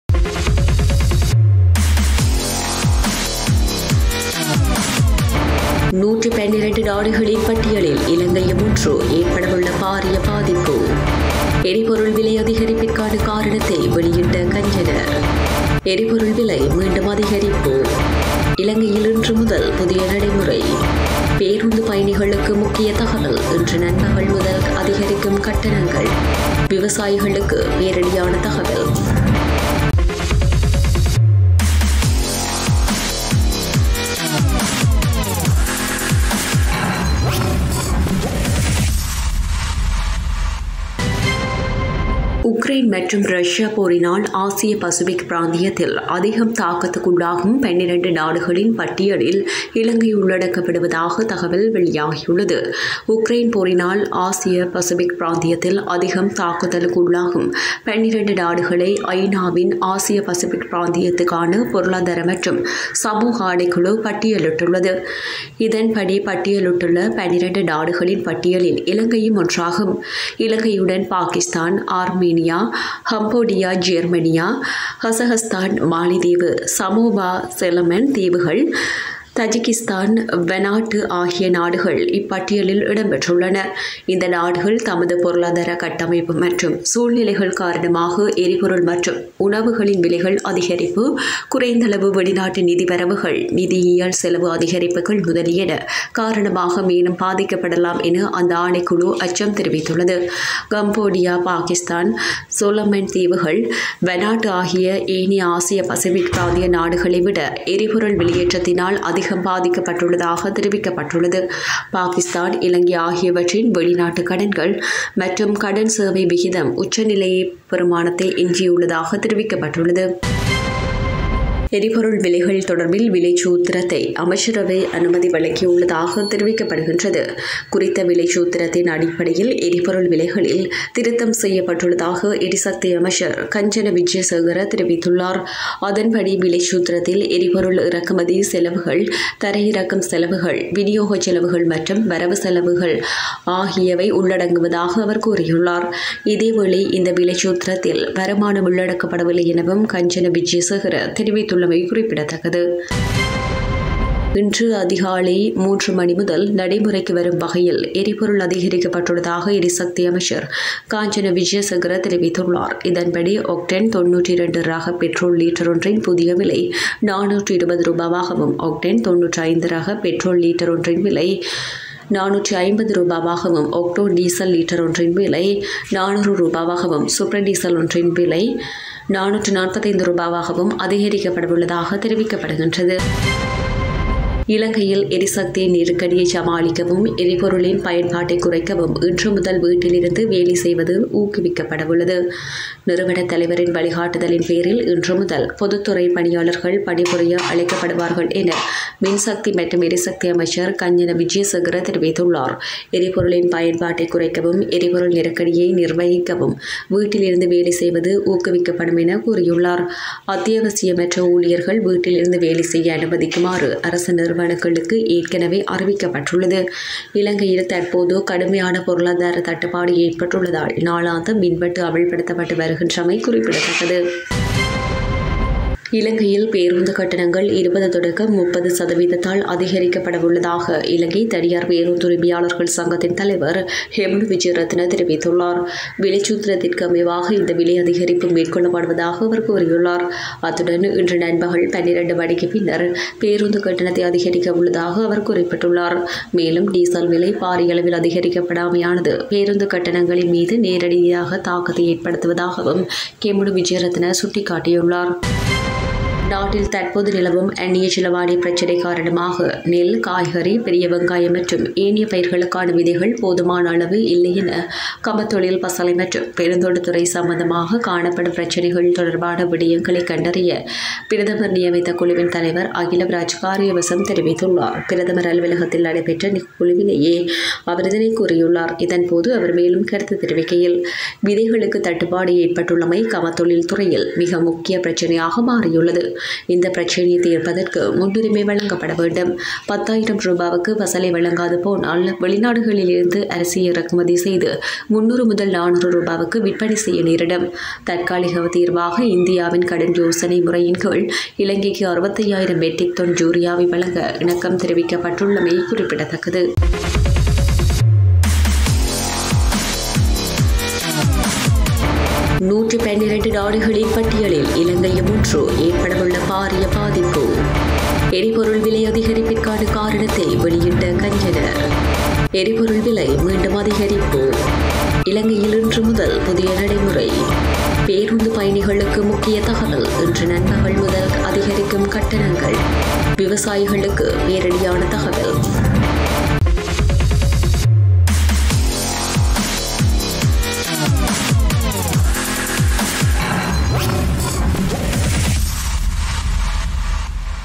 காலை நேர பிரதான செய்திகள் | 24.05.2022 | Today Sri Lanka Tamil News | T24.News - Main Tamil News.
-நாடுகளின் பட்டியலில் இலங்கையும் ஒன்று!
-எரிபொருள் விலை அதிகரிப்பிற்கான காரணத்தை வெளியிட்ட கஞ்சன
-எரிபொருள் விலை மீண்டும் அதிகரிப்பு
-இலங்கையில் இன்று முதல் புதிய நடைமுறை
-இன்று நண்பகல் முதல் அதிகரிக்கும் கட்டணங்கள்
-விவசாயிகளுக்கு பேரிடியான தகவல்..